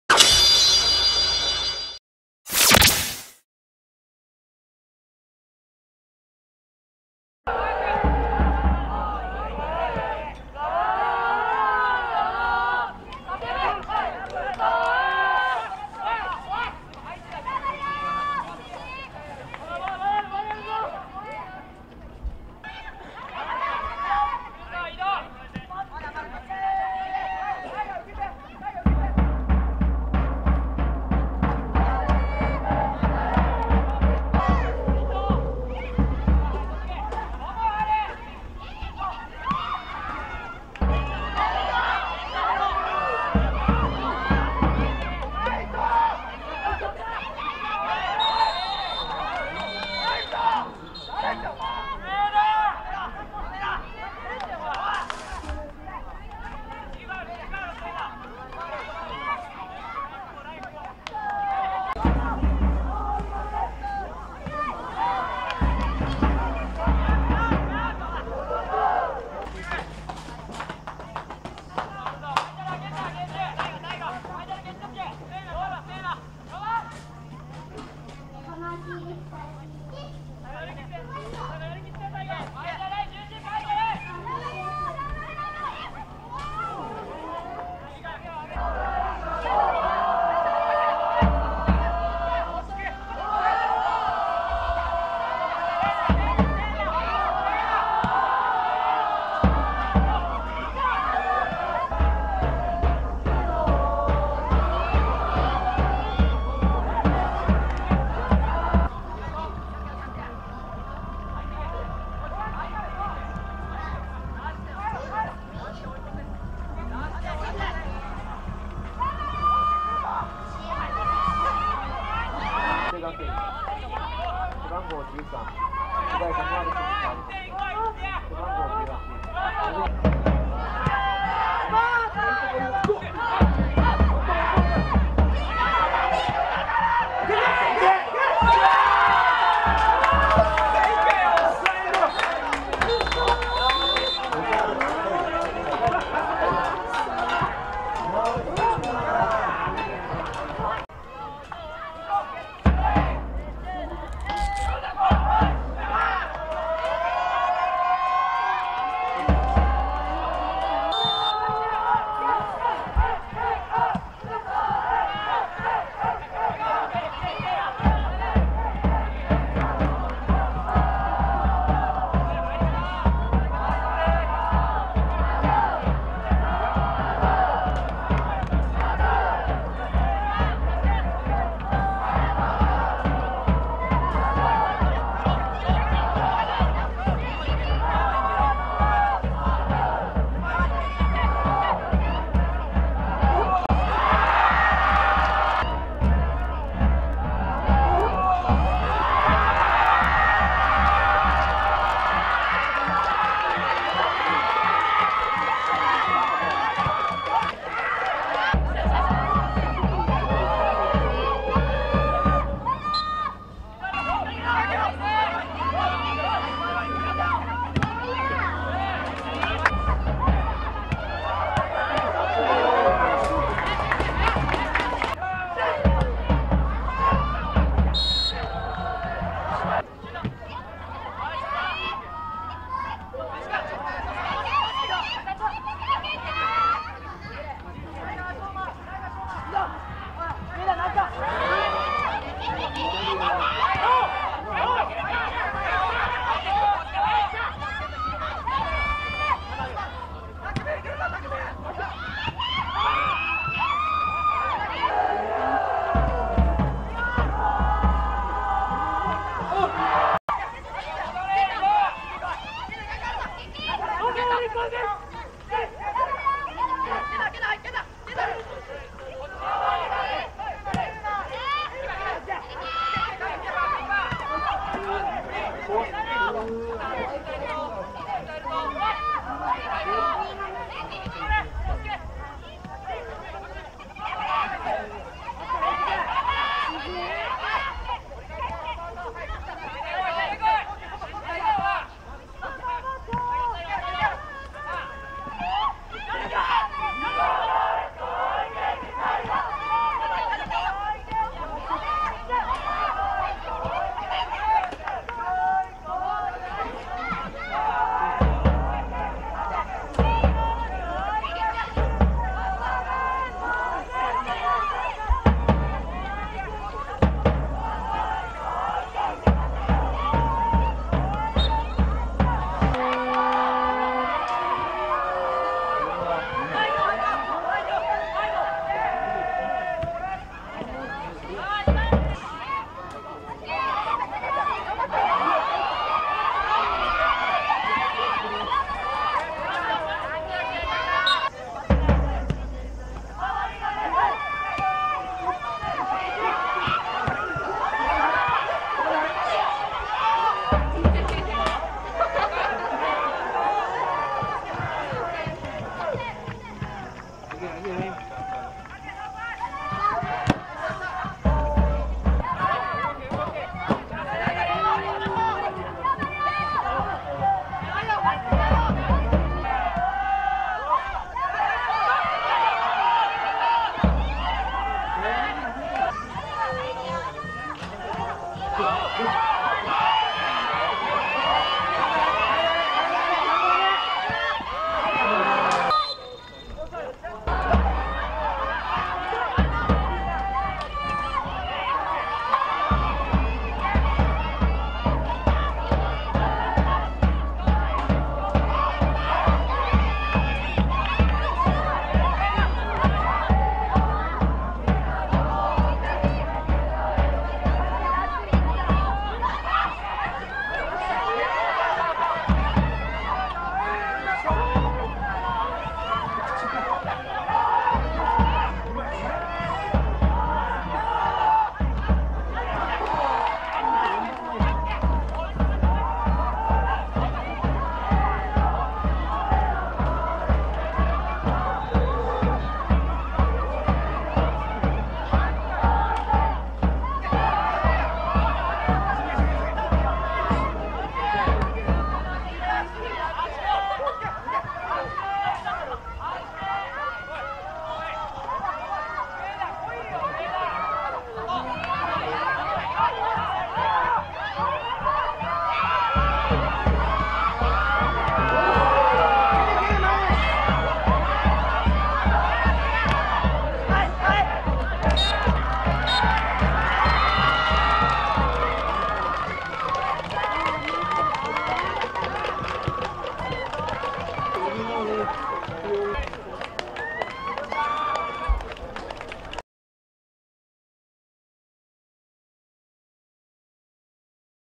第４７回九州中学サッカー競技大会２回戦ルーテル中vs宮崎日大中
鹿児島２位の伊敷台中に2-1で勝利したルーテル（熊本１位）。福岡２位の浅川中に3-0で勝利した宮崎日大中（宮崎１位）の対戦。
前半0-0でご部の戦いだったが、後半開始４０秒で宮崎日大中が得点。